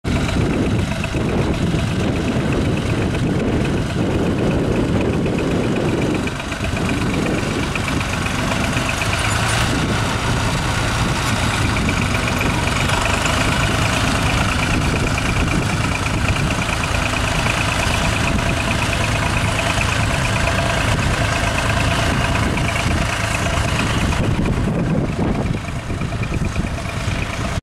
Australian Ploughing Championship 2022 Part 10
Another clip from the Australian Ploughing Championships held at "Mount Ireh Estate" Longford on 25th June 2022. In this clip we have Colin Berne driving a Massey Ferguson 135 with a 2 furrow Kverneland competition mouldboard plough in action.